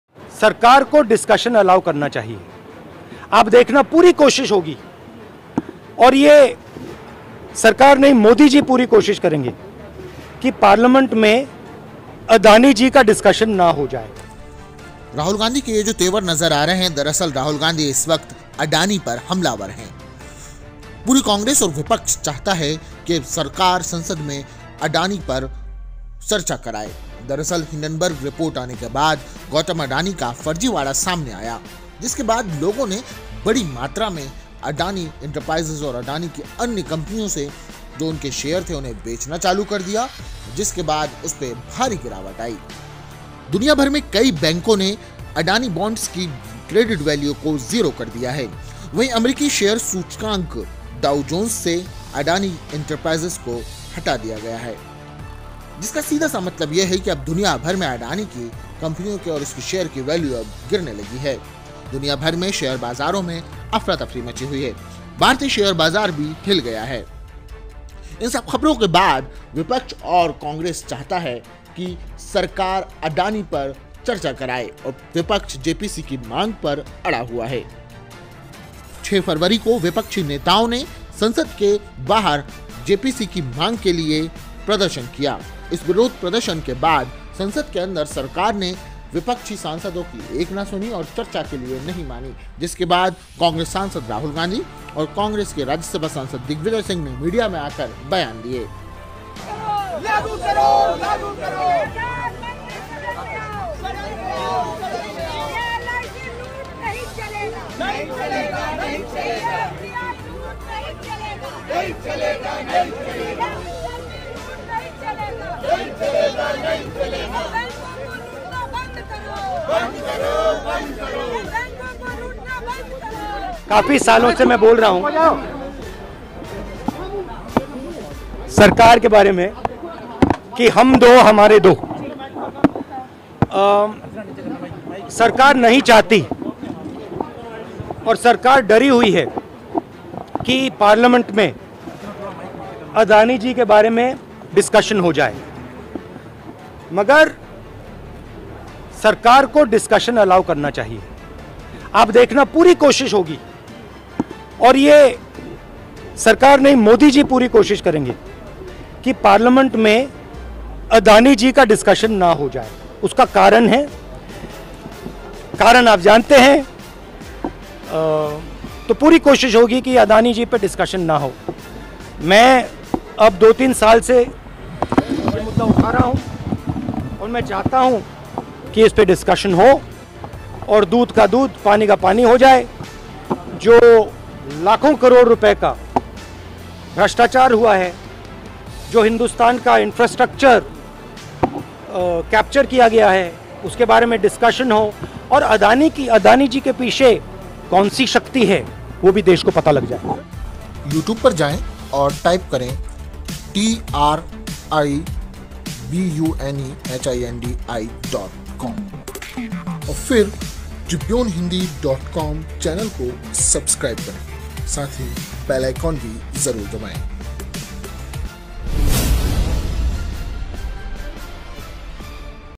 विपक्ष चाहता है कि सरकार Gautam Adani पर संसद में चर्चा कराये , और JPC का गठन कर जांच कराई जाए
अमेरिकी रिसर्च कंपनी हिंडनबर्ग रिसर्च की रिपोर्ट के बाद गौतम अडानी के शेयरों में भारी गिरावट देखी गई। इस रिसर्च रिपोर्ट में बताया गया था कि गौतम अडानी की कंपनी के शेयर ओवरवैल्यूड हैं। उनकी कंपनियों में शेल कंपनियों के ज़रिए निवेश किया जा रहा है ।
जब यह रिपोर्ट सामने आई है, इस फर्जीवाड़े की बात लोगों को पता चली तो दुनिया भर में गौतम अडानी के शेयरों में भारी गिरावट देखने को मिली है।

इस रिपोर्ट के आने के बाद भारत ने भी शेयर मार्केट जमकर गिर गया और गौतम अडानी की इनकम में भारी कमी देखी गई। वह दो नंबर से सीधे बाईसवें नंबर पर पहुंच गए ।

इसी के साथ विपक्ष भी चाहता है कि गौतम अदानी पर केंद्र सरकार JPC गठित कर जांच कर और संसद पर उस पर चर्चा हो ।  मगर केंद्र सरकार गौतम अडानी पर किसी भी तरह की चर्चा से दूर भाग रही है ।

कांग्रेस नेता राहुल गांधी ने इस पर अपना बयान दिया है और कहा कि किस तरह सरकार चर्चा से दूर भाग रही है।